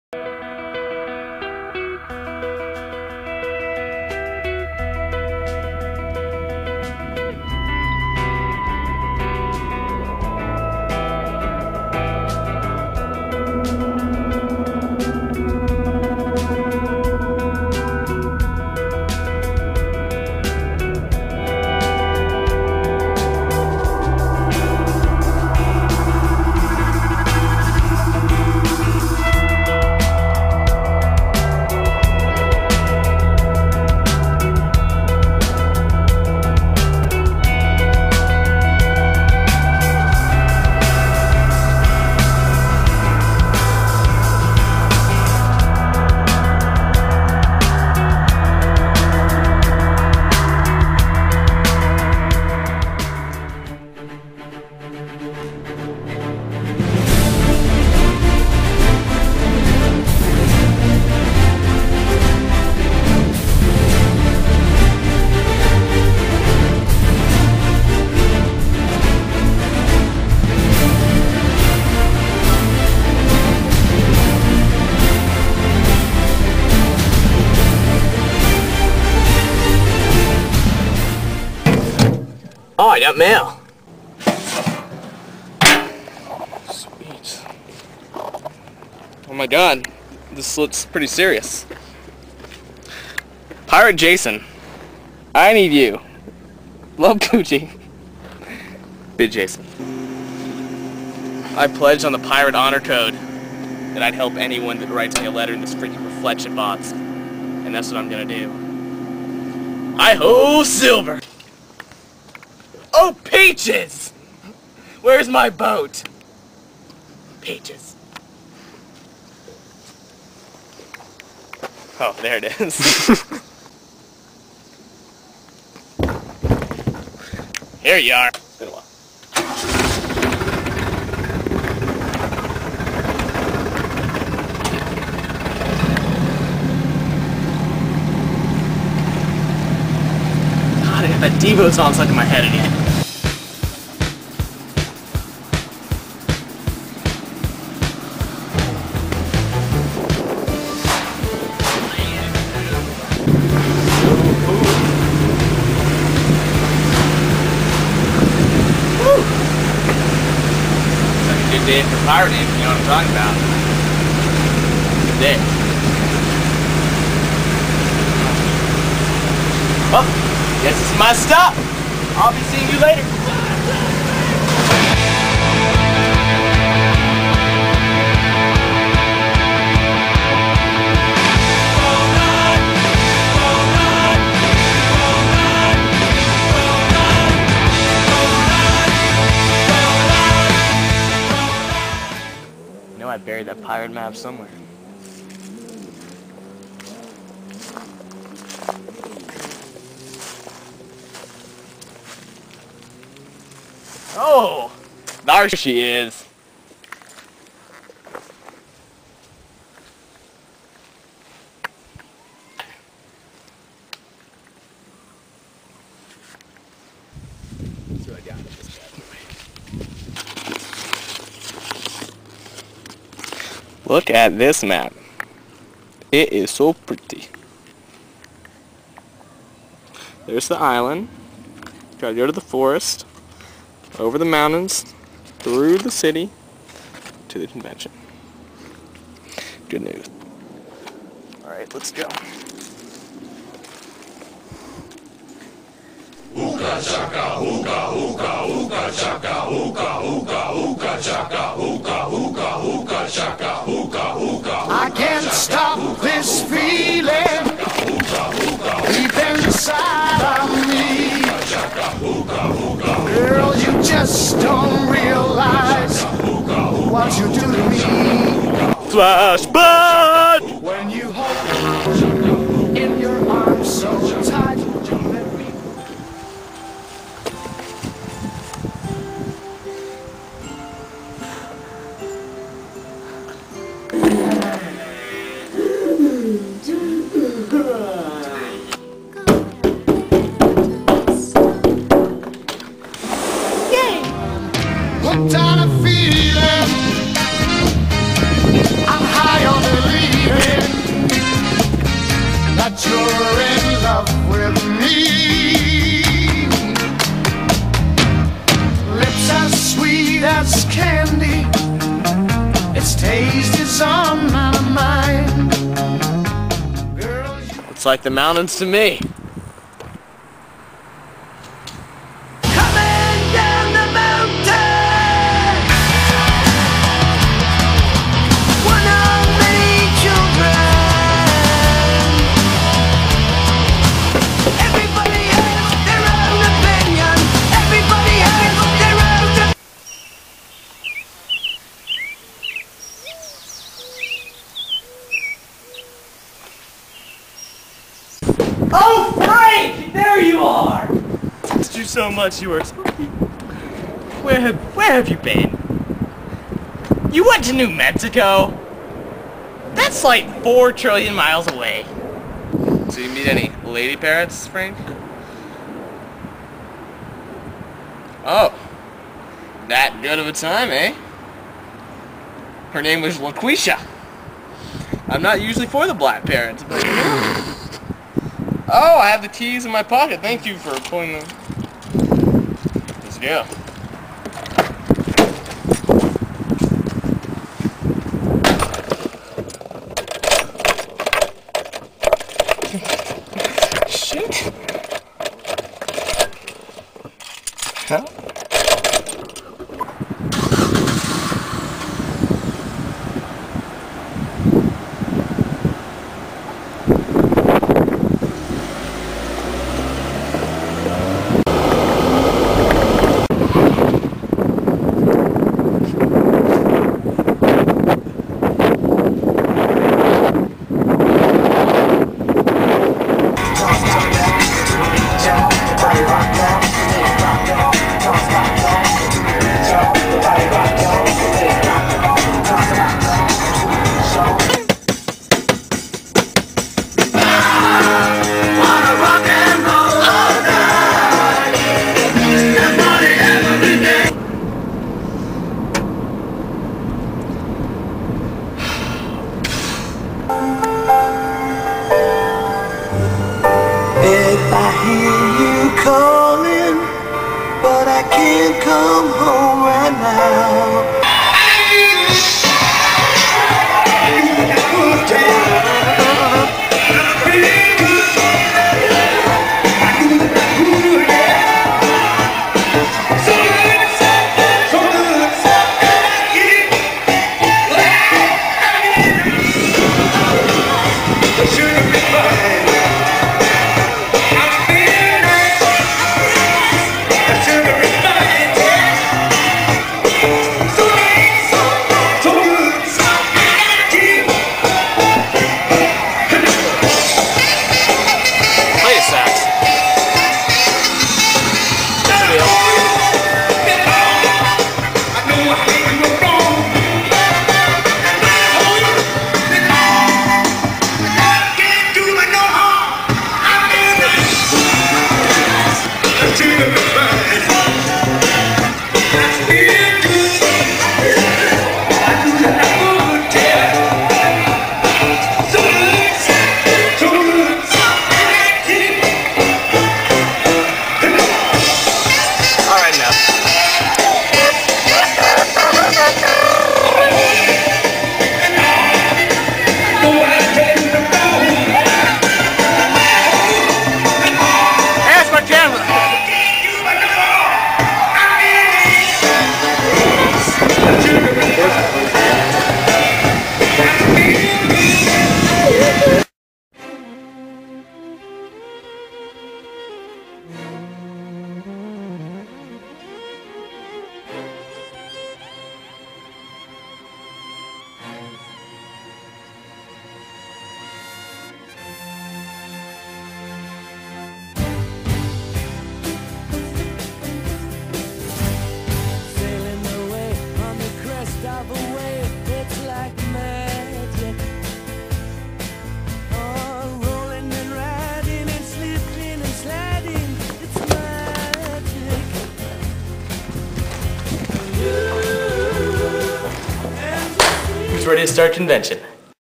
Pirate Video
2005 Fall Sub Regional Convention  in Charlotte started a revolution in USY with movies for the introduction.